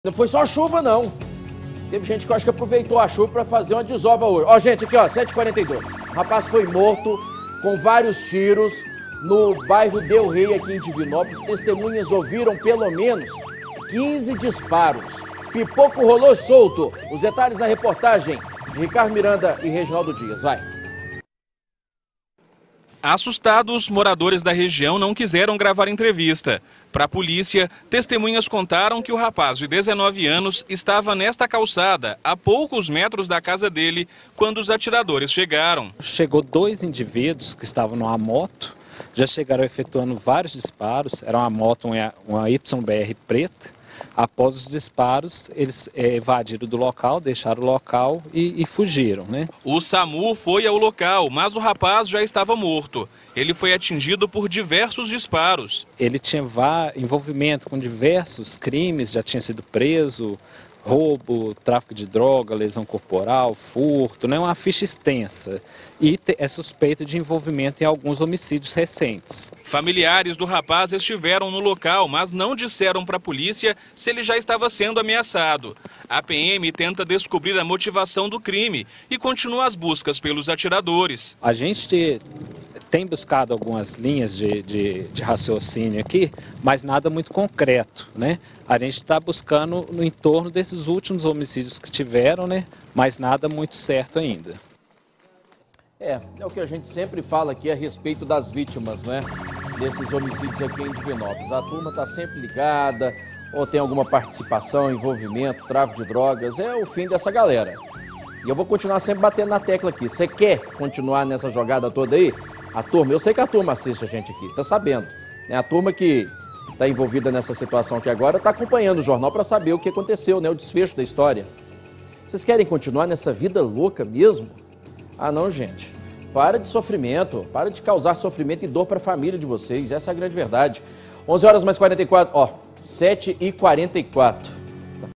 Rapaz é morto com vários tiros no Bairro Del Rey em Divinópolis
De acordo com a PM, o jovem de 19 anos tem diversas passagens policiais.